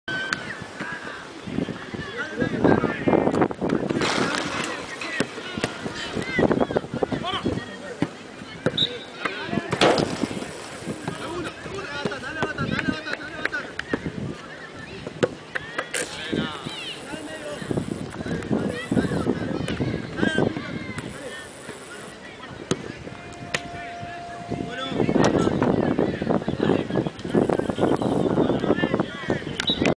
A.Previo8.MPG
Acondicionamiento Previo ACB (Asociación Coronel Brandsen) Campeón Liga Amateur Platense de Fútbol. Torneo Clausura 2007.